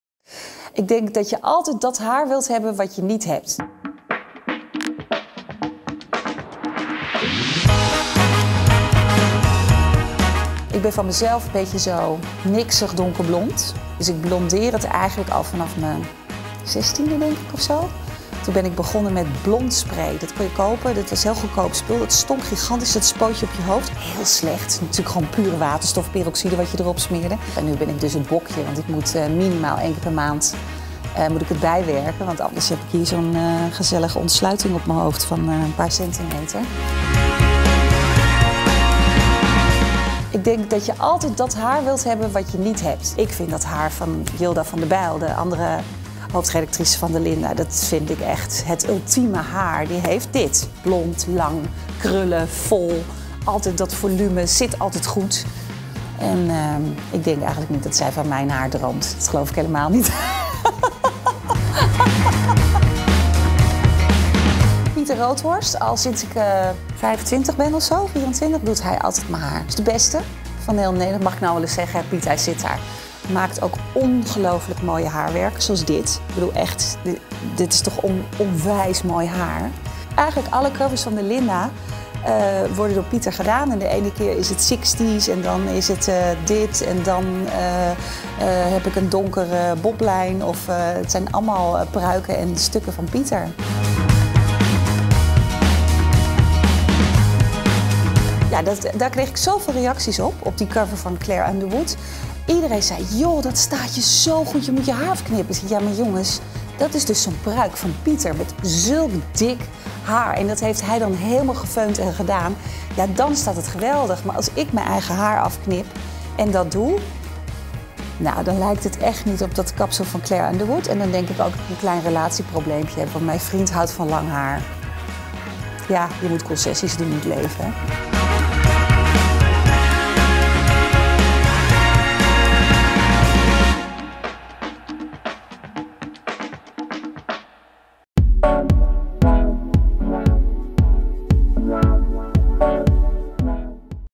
Linda de Mol over echt, nep, prachtig en rottig haar || BACKSTAGE VIDEO
In de nieuwe LINDA. 154 'HAIR' biecht Linda de Mol op dat ze op haar zestiende al begon met blonderen: 'En nu ben ik dus het bokje, want nu moet ik het minimaal één keer per maand bijwerken.' Linda kreeg veel positieve reacties op de LINDA. cover van 'LIEVER SERIES DAN SEX' (bekijk 'm hier). Daar had ze het kapsel van Claire Underwood: 'Maar dat is dus een pruik. Als ik mijn haar afknip en dat doe… Dan lijkt het écht niet op het kapsel van Claire Underwood en heb ik een klein relatieprobleempje. Mijn vriend houdt van lang haar.'